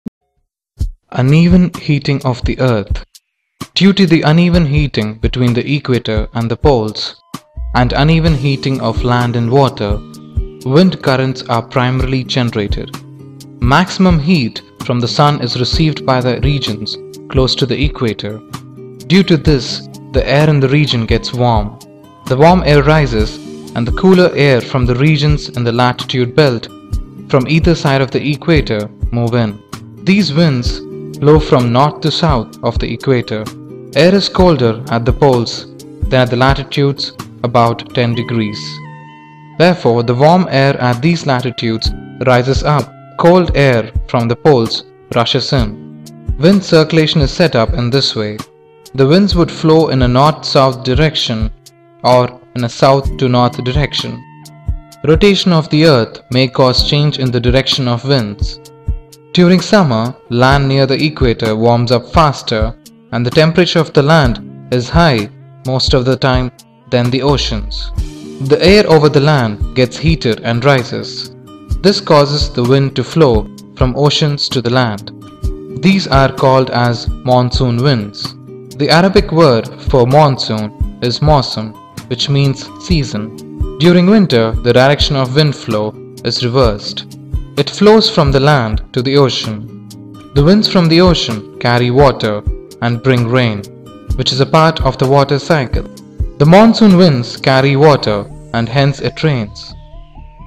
Uneven heating on the Earth's surface -Elementary Science
This is an Elementary Science Video for kids which talks about uneven heating on the Earth's surface and the resulting variations in temperature and climate on the Earth.